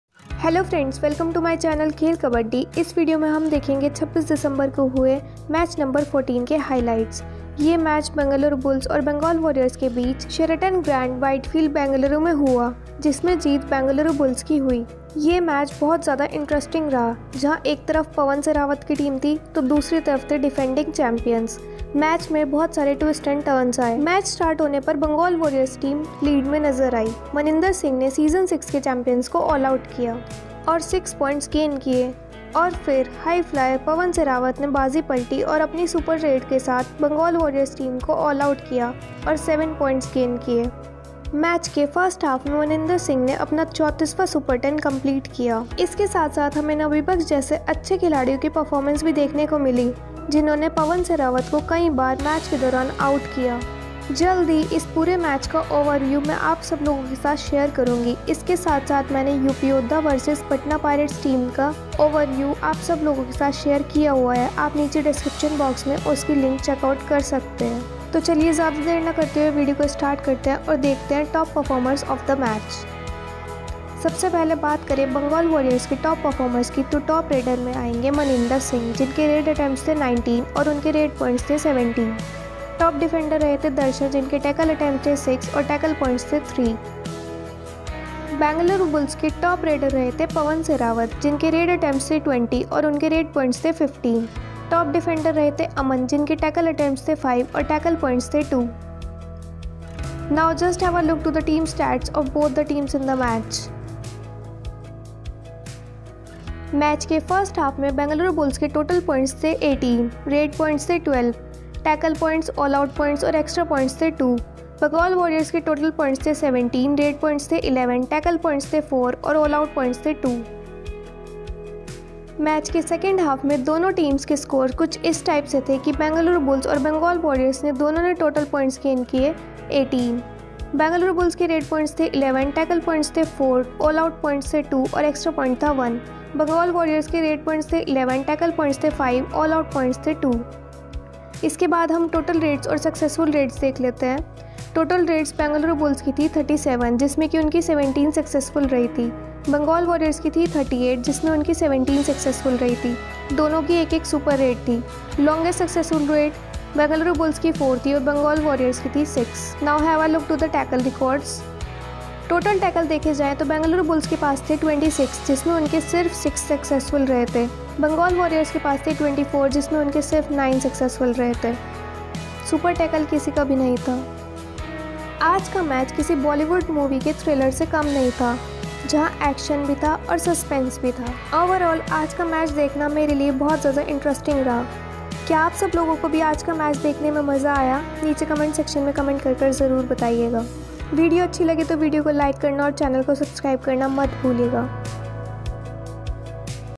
Bengaluru Bulls Vs Bengal Warriors | Highlights | 26 December 2021 | Pro Kabaddi
Bengaluru Bulls Vs Bengal Warriors and the point table

 KHEL KABADDI
NOTE :-
This video is made by me for knowledgeable purposes. The graphics added in this video and animations made are totally edited by me even the voice over is also done by myself only.